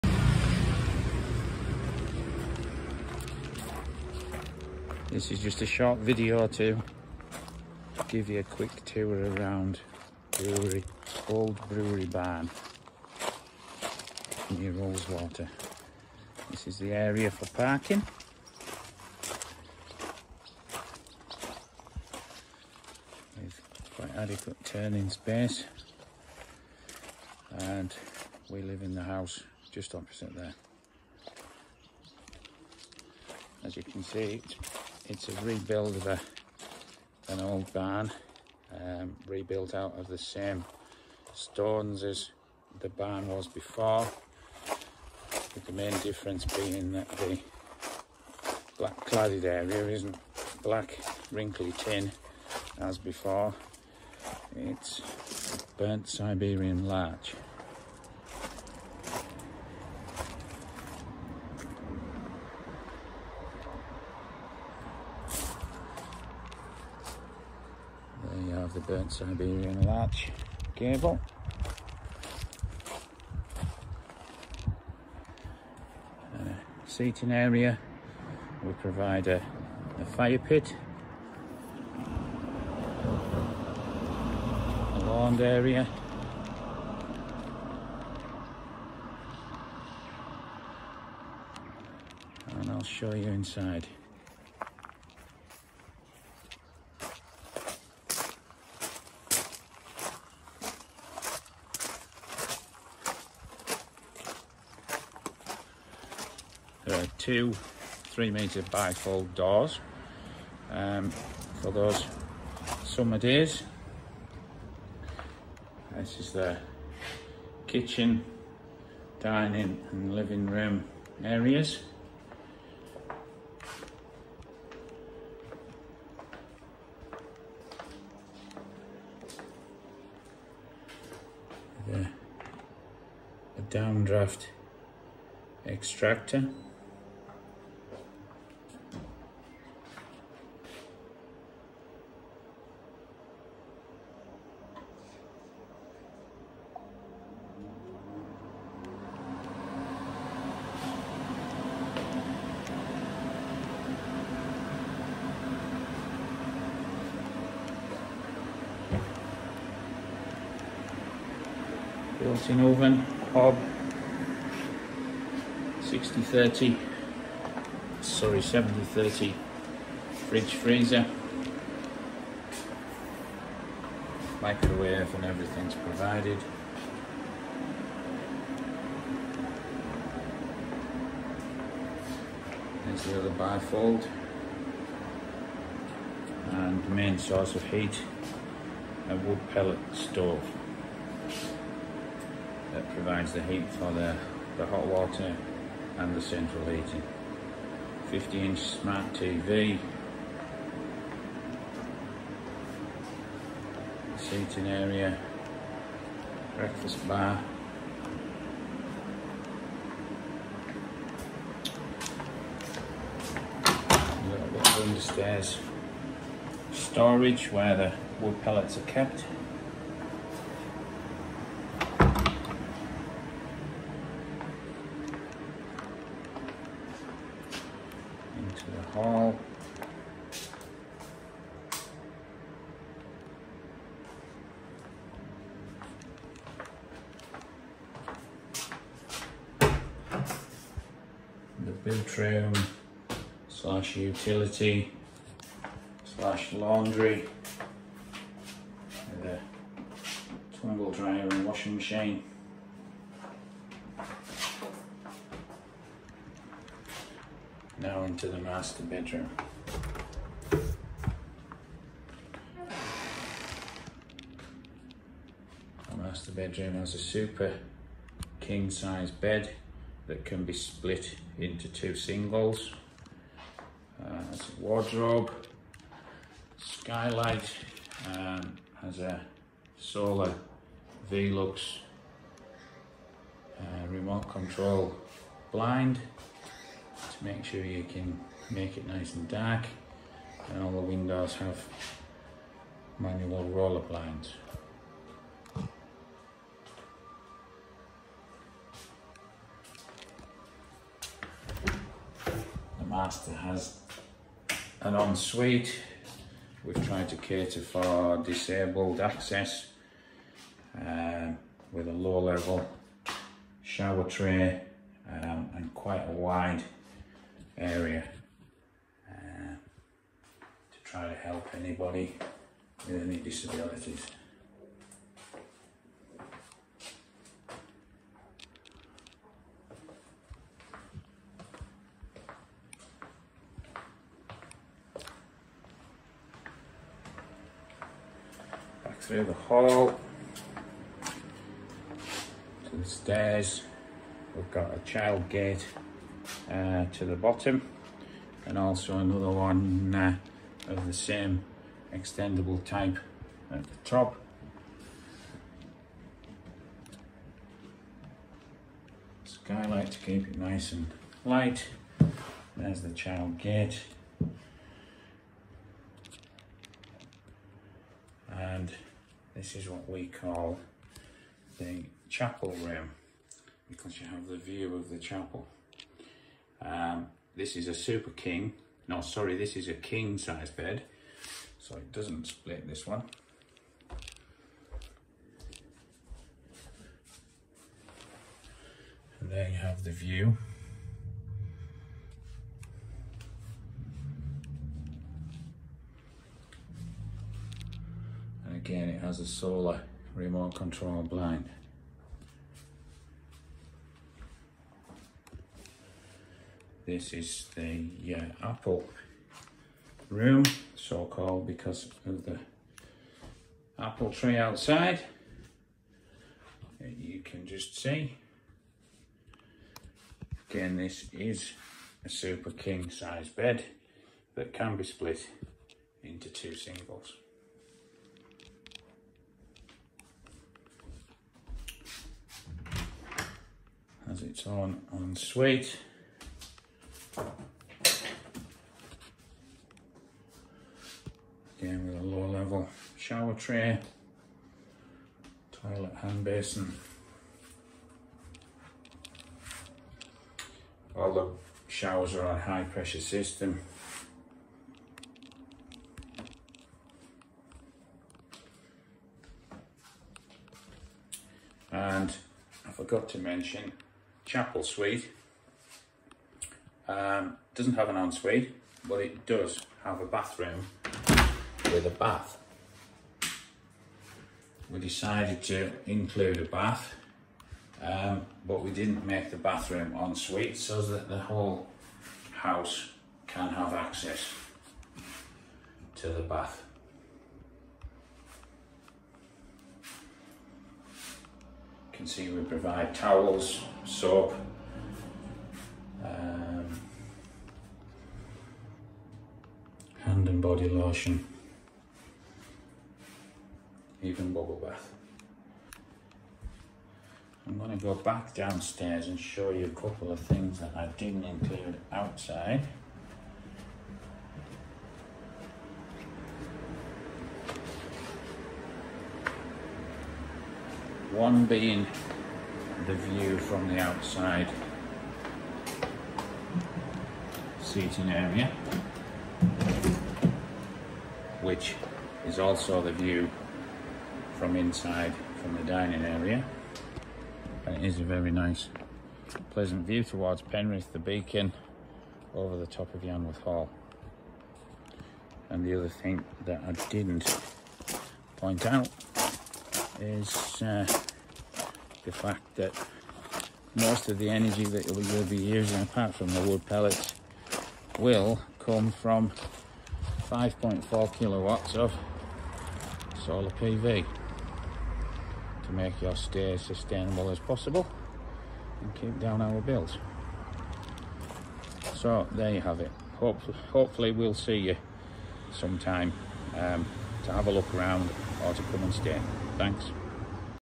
Old Brewery Barn, Ullswater
Old Brewery Barn, near Ullswater, holiday house. Sleeps six, three bathrooms, open plan living and lots of outdoor space. Find us on Airbnb, VRBO and booking.com